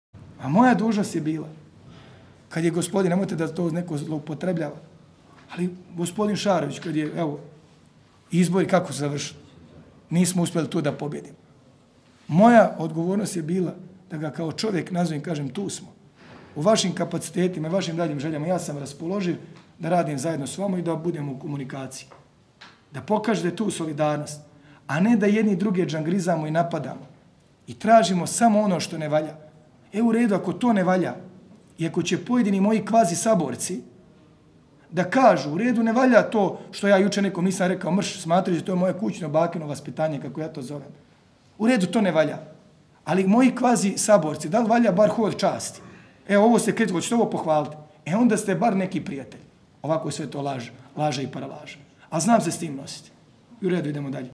Stanivuković o optužbama da je izdao oporbu
Gradonačelnik Draško Stanivuković o jučerašnjem ručku s predsjednikom Gradskog odbora SNSD-a Vladom Đajićem i optužbama da je izdao oporbu u RS.